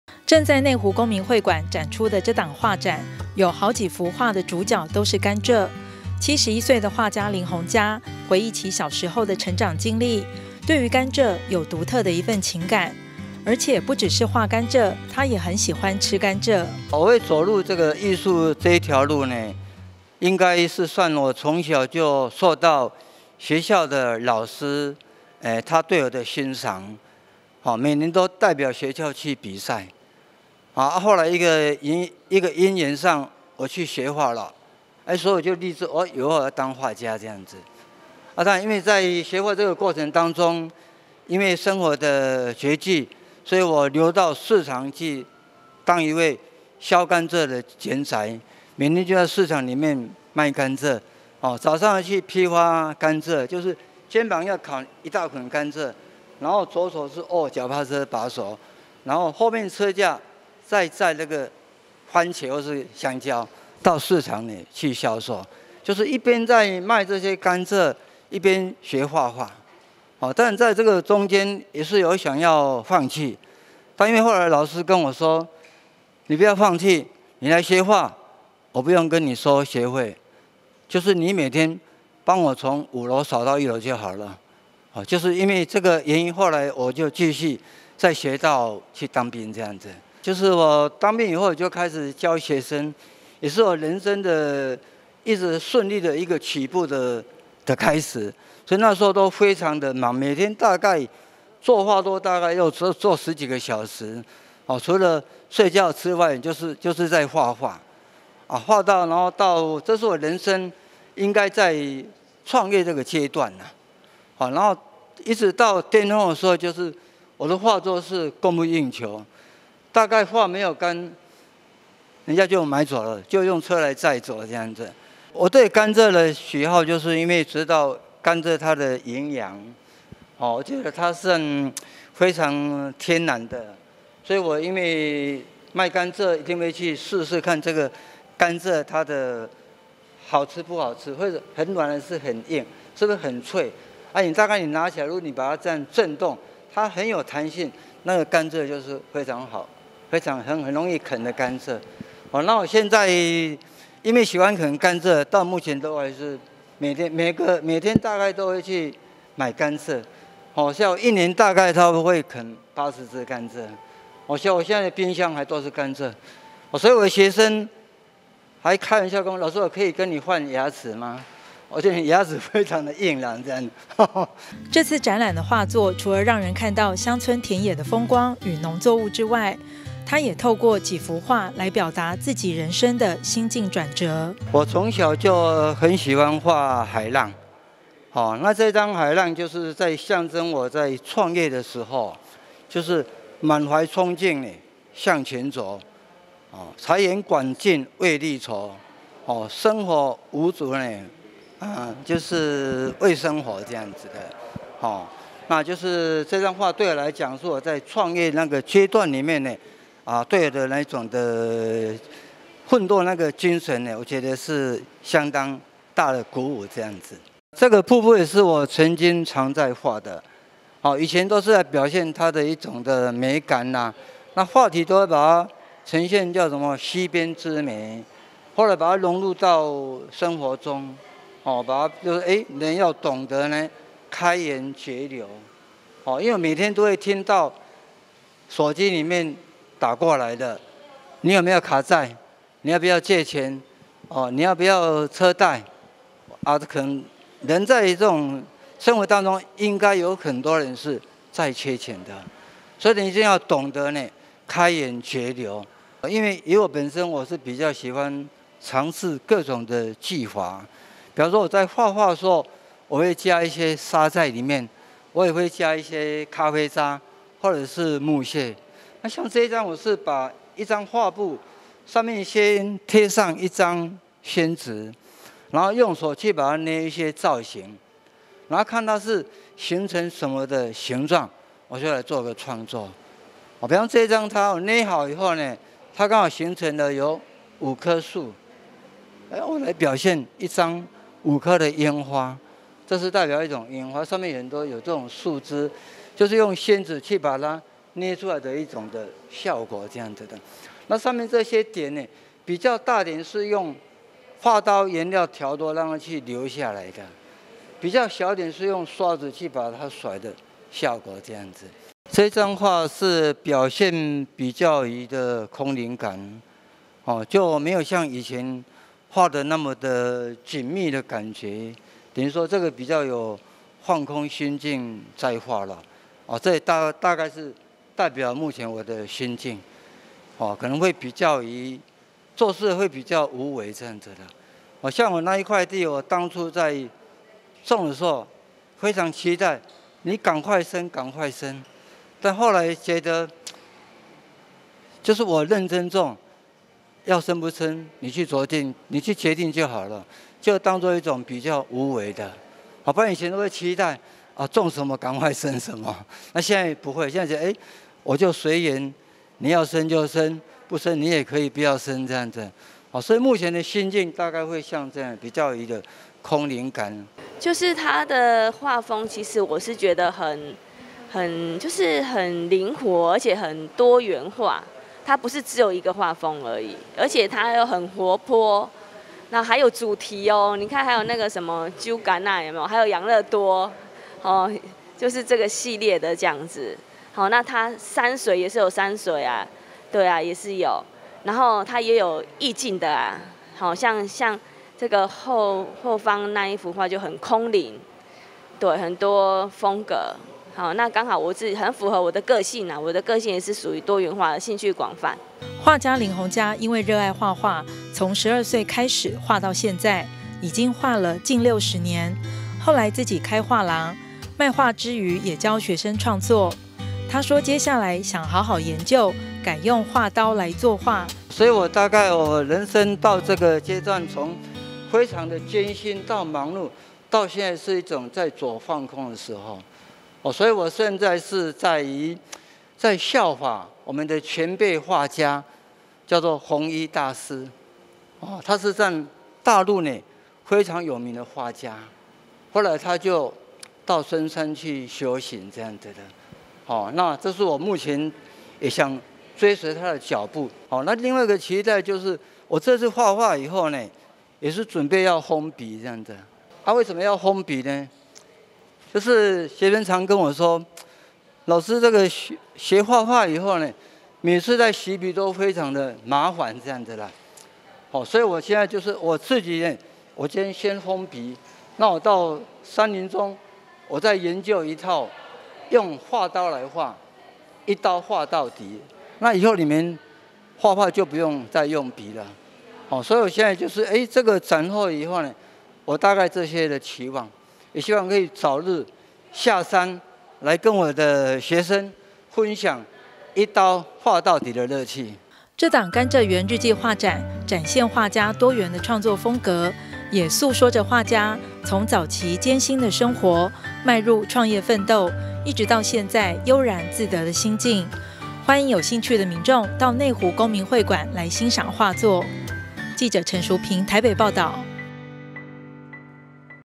內湖|畫展|林鴻嘉甘蔗園日記畫展 展現人生經歷
正在內湖公民會館展出的這檔畫展，有好幾幅畫的主角都是甘蔗，71歲的畫家林鴻嘉回憶起小時候的成長經歷，對於甘蔗有獨特的一份情感。而且不只是畫甘蔗，他也很喜歡吃甘蔗。

這次展覽的畫作除了讓人看到鄉村田野的風光與農作物之外，他也透過幾幅畫來表達自己人生的心境轉折，另外他也勇於嘗試用不同的技法來作畫，現場可以欣賞到多元的畫風。

畫家林鴻嘉，因為熱愛畫畫，從12歲開始畫到現在，已經畫了近60年，後來自己開畫廊，賣畫之餘也教學生創作。他說接下來想好好研究用畫刀來作畫。
這檔「甘蔗園日記」畫展，展現畫家多元的創作風格也訴說著畫家從早期艱辛的生活邁入創業奮鬥一直到現在悠然自得的心境，即日起展出到12/21，歡迎有興趣的民眾到內湖公民會館來欣賞畫作。